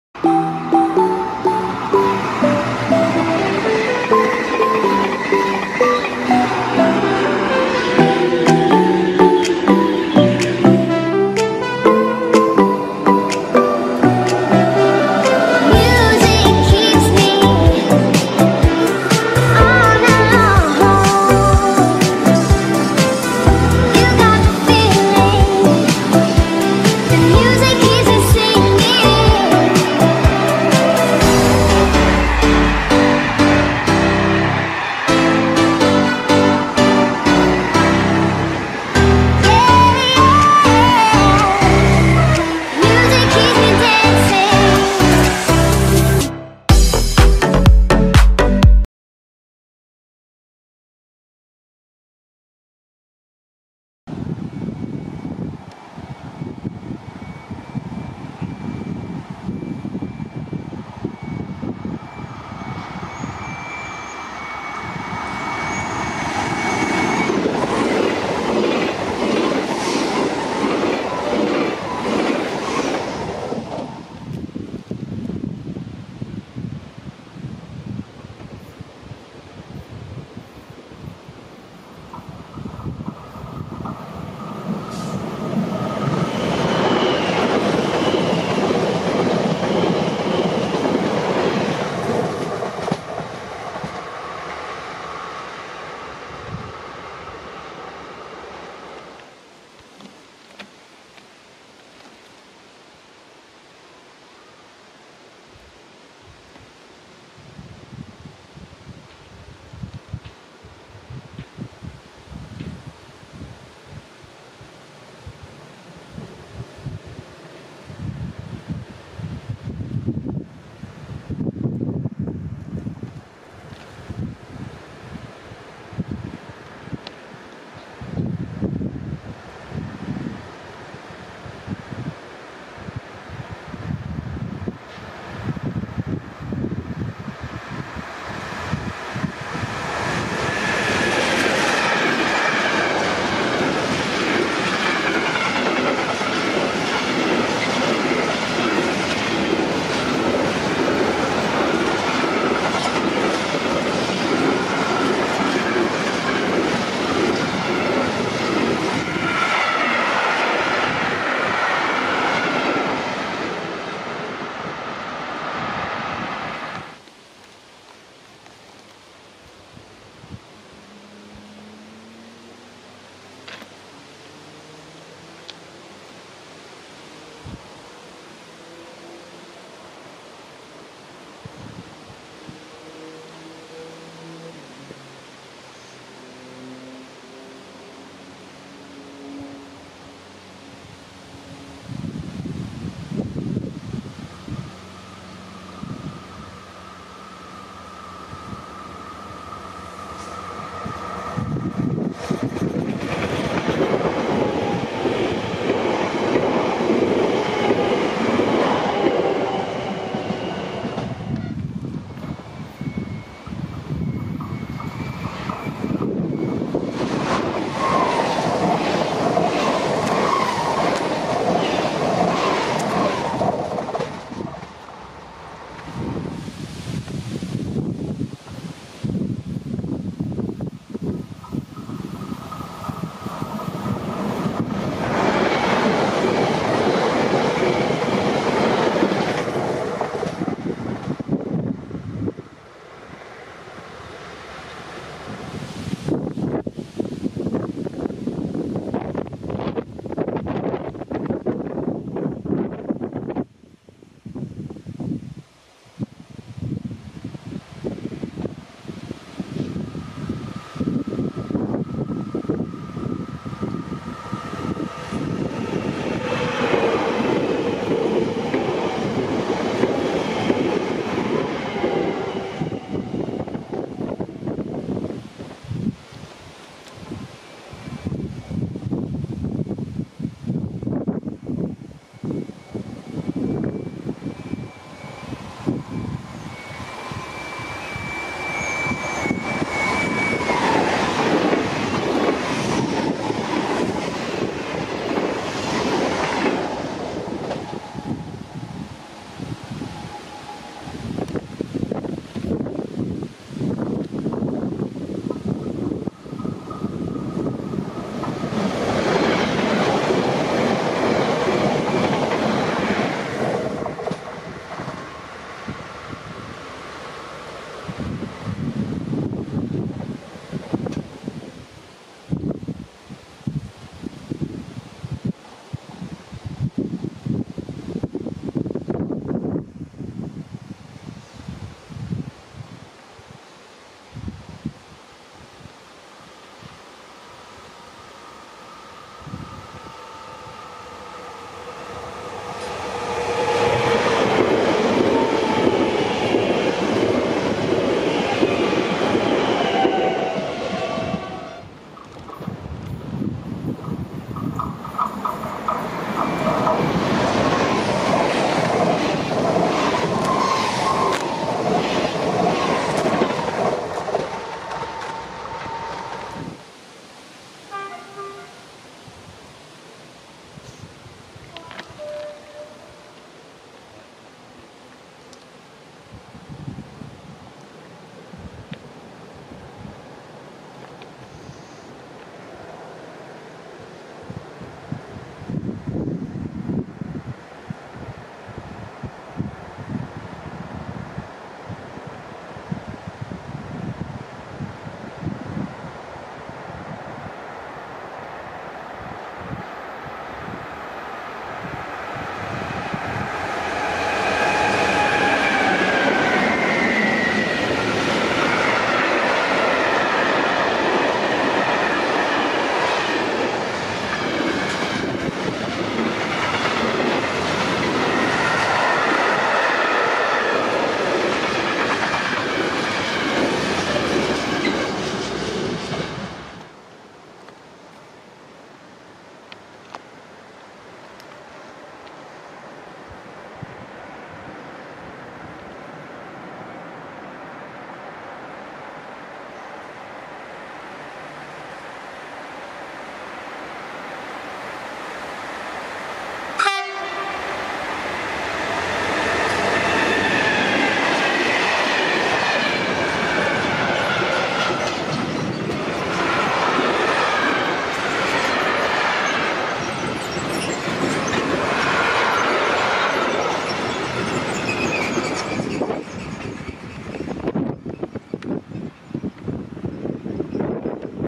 Fret et transilien au PN8 à Vilenoy
Bonjour à tous on se retrouve aujourd'hui pour une nouvelle vidéo d'un petit spot au PN8 à Villenoy !
Bonne vidéo !